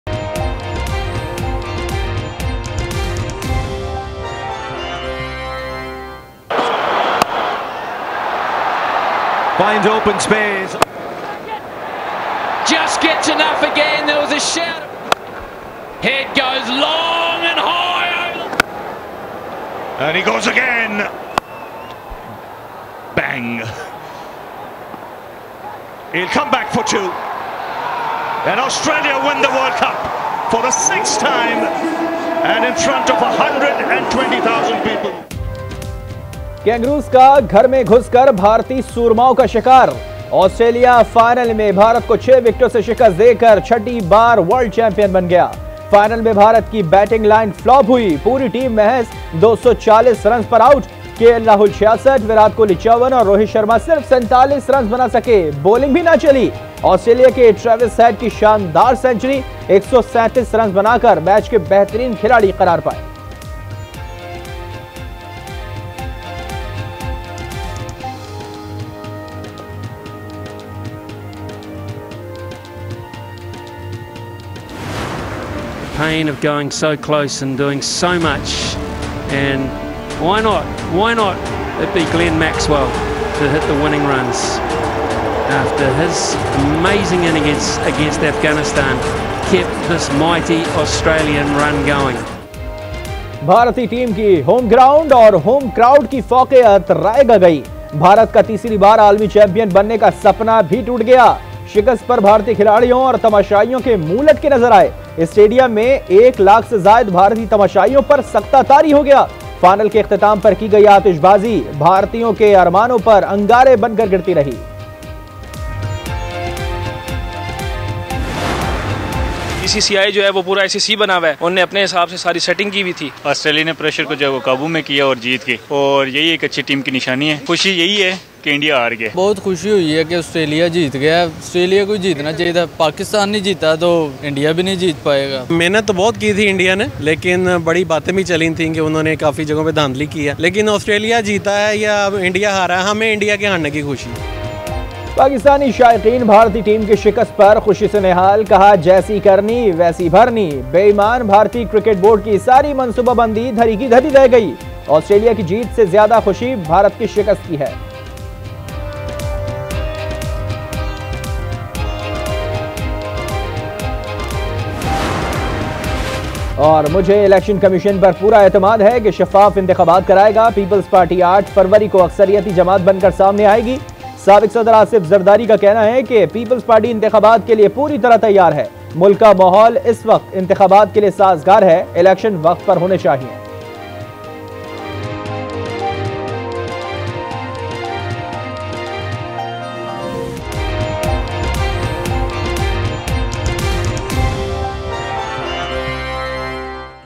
Australia beat India to win 6th World Cup - 10PM Headlines - Aaj News
10PM Headlines: Australia beat India to win 6th World Cup - Aaj News

Spot Light With Munizae Jahangir:
shorturl.at/gwFU6